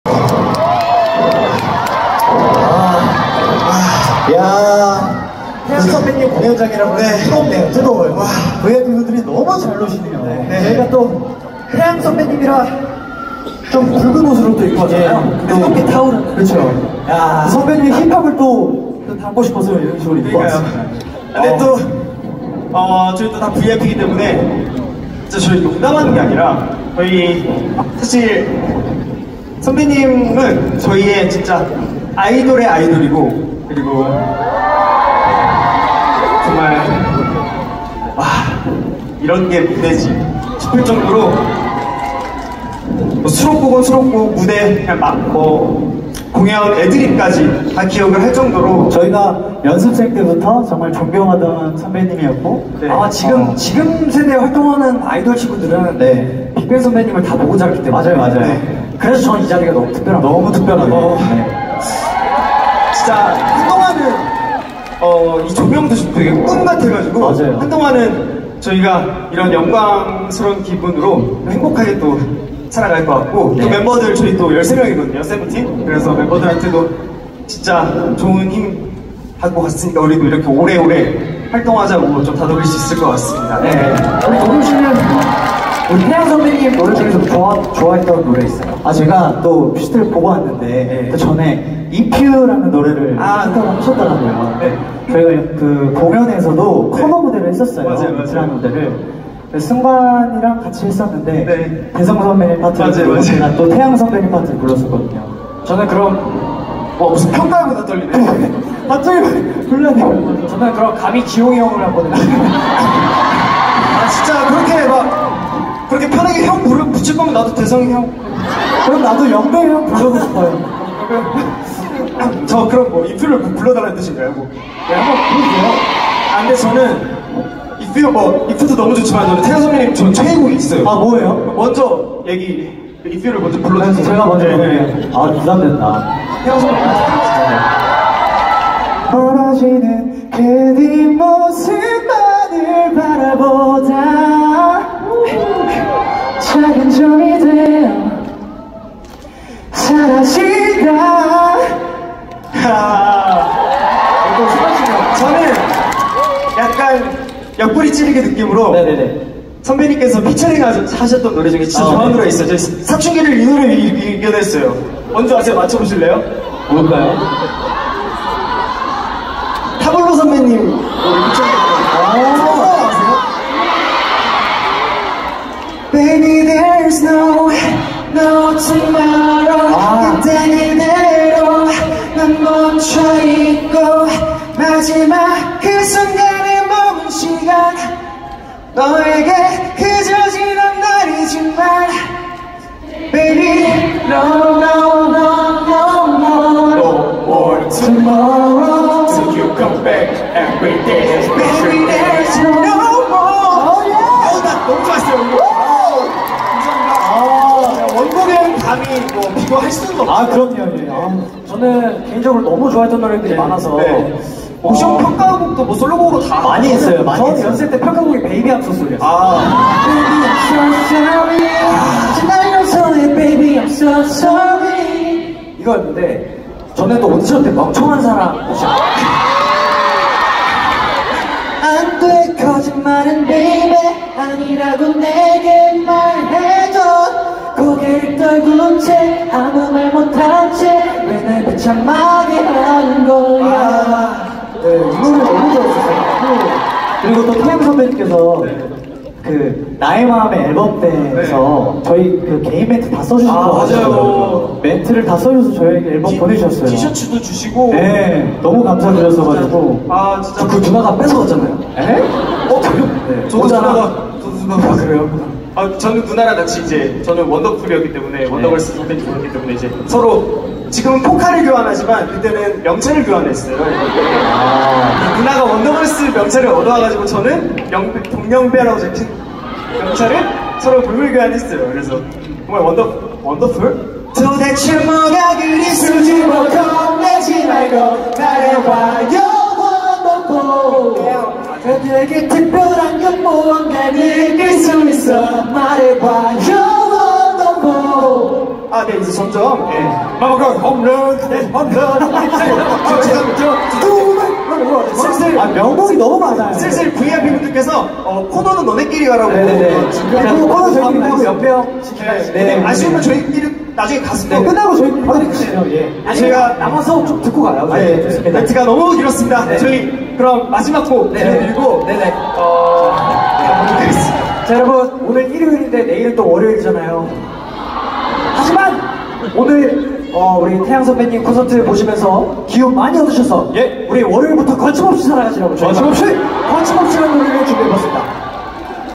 [ENG SUB] 20240901 BSS Seventeen 세븐틴 부석순 talk about how they like BigBang at THE LIGHT YEAR Concert
4K Entertainment News Report

BSS cover many classic songs too

English subtitles by Google translation and it may not be accurate